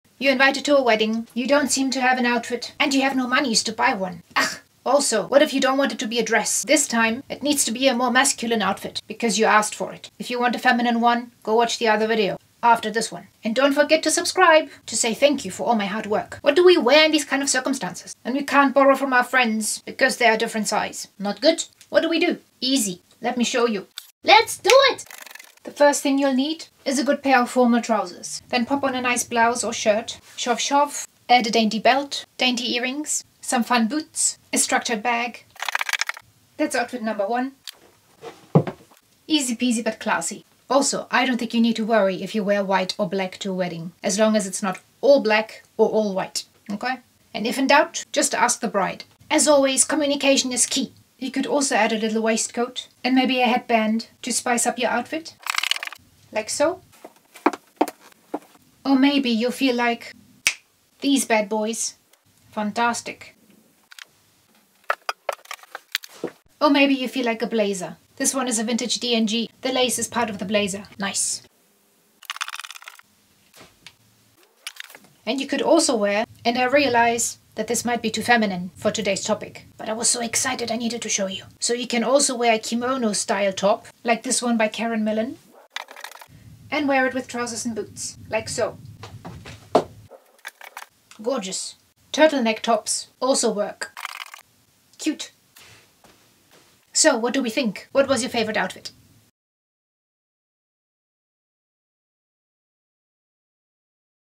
Slightly more masculine wedding guest outfit for when you're broke
🌷 Support me:
Thanks for considering becoming a Galaxy Magpie!

You asked me to style some wedding guest outfits for women who like more masculine looks so here are some.